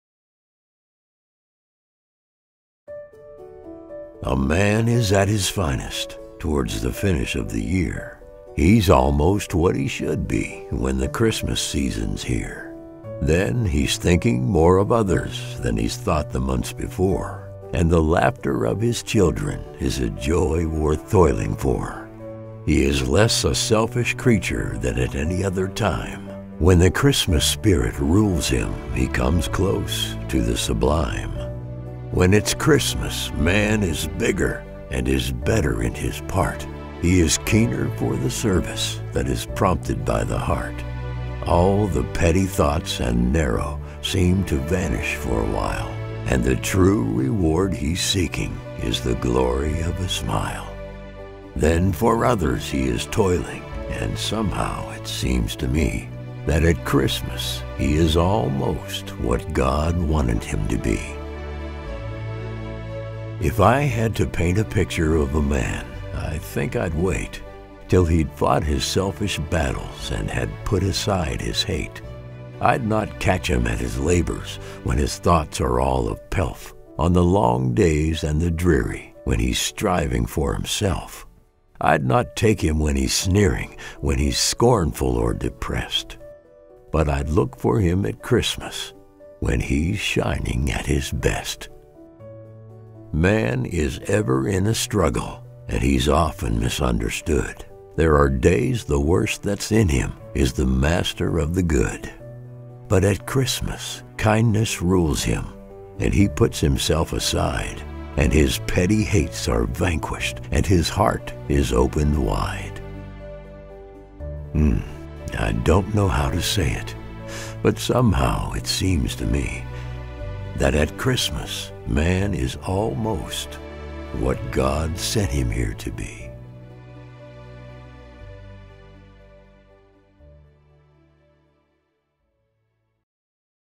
AT CHRISTMAS, by Edgar A. Guest
My 2023 narration of a Christmas poem by Edgar A. Guest.
Written for ALL men everywhere... and the women & children who love them.